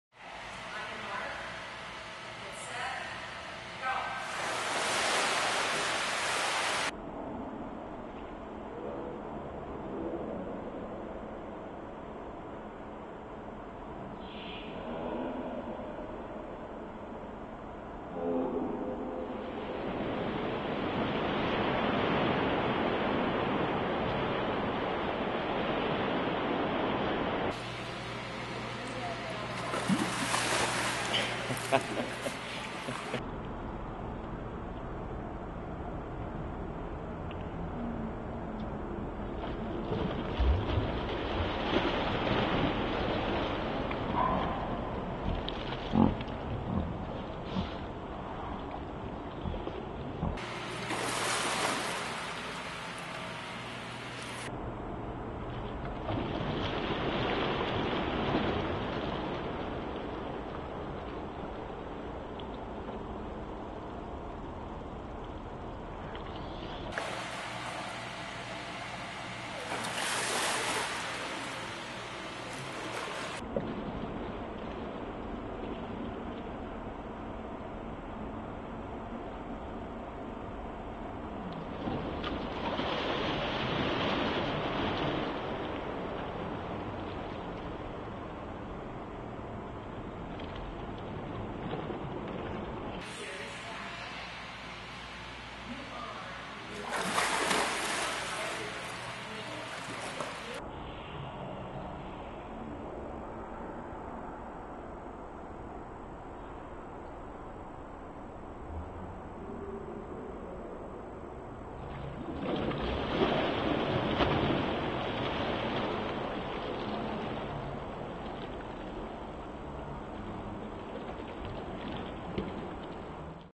Back Crawl Starts
Working on his back crawl starts.